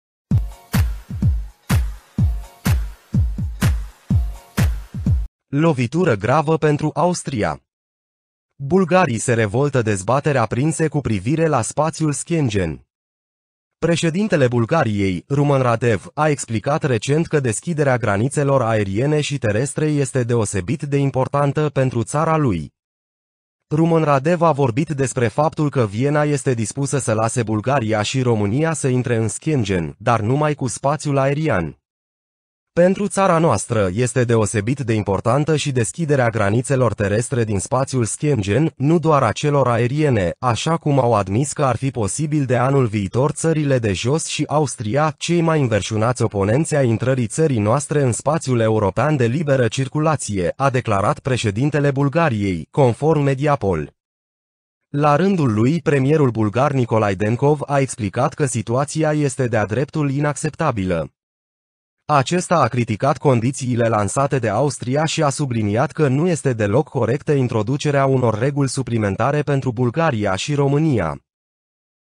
Lovitură gravă pentru Austria! Bulgarii se revoltă
Dezbateri aprinse cu privire la spațiul Schengen! Președintele Bulgariei, Rumen Radev, a explicat recent că deschiderea granițelor aeriene și terestre este „deosebit de importantă” pentru țara lui.